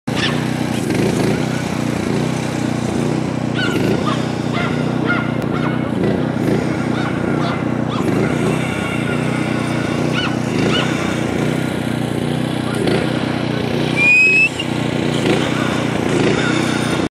Lure Coursing 1st Run Ever Siberian Husky June 14th, 2013
Towards the end of his first run he lost sight of the lure and ran off course but he came right to me when I called him.